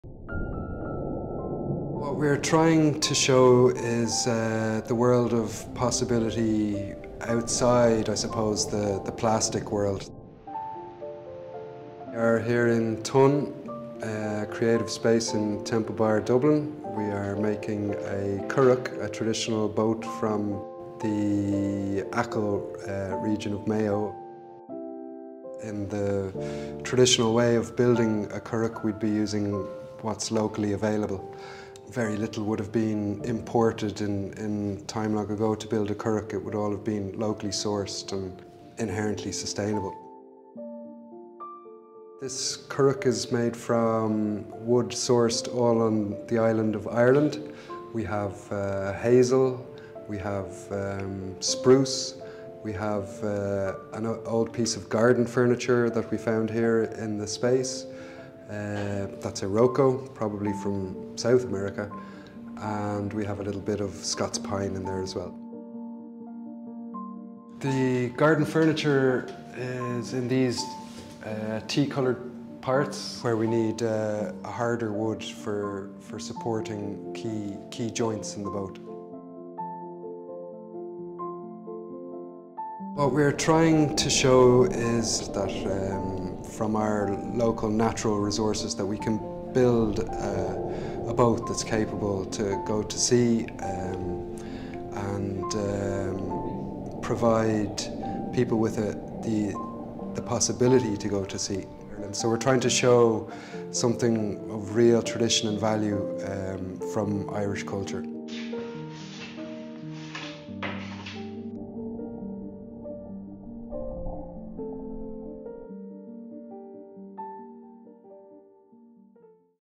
Recycled garden furniture part of sustainable currach build in Dublin's Temple Bar
An artist has built a currach made entirely from wood sourced and salvaged in Ireland - with recycled garden furniture one of the boat's key components. Mark Redden, who has been building currachs for almost 20 years, says the project in Dublin's Temple Bar connects to ideas of reuse and repair, as well as the joy of being at sea.